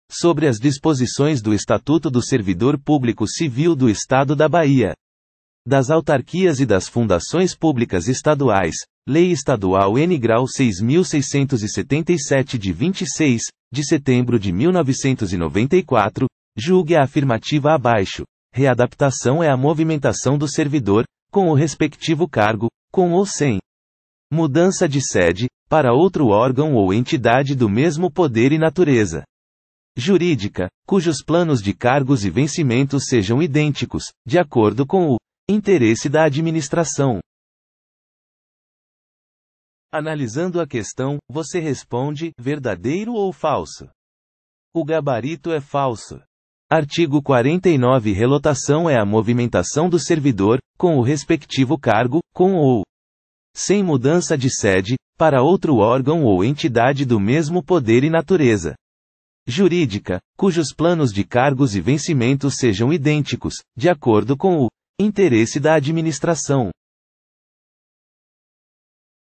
QScon Direito: [Art. 49, Lei Estadual 6.677/94 - BA] IBFC 2015 - Analista de Registro de Comércio (S
Art. 49, Lei Estadual 6.677/94 BA | IBFC 2015 | Analista de Registro de Comércio | (SAEBBA)

Sobre as disposições do Estatuto do Servidor Público Civil do Estado da Bahia,. das Autarquias e das Fundações Públicas Estaduais (Lei estadual n° 6.677 de 26. de setembro de 1994), julgue a afirmativa abaixo: Readaptação é a movimentação do servidor, com o respectivo cargo, com ou sem. mudança de sede, para outro órgão ou entidade do mesmo Poder e natureza. jurídica, cujos planos de cargos e vencimentos sejam idênticos, de acordo com o. interesse da administração.

Gabarito: Falso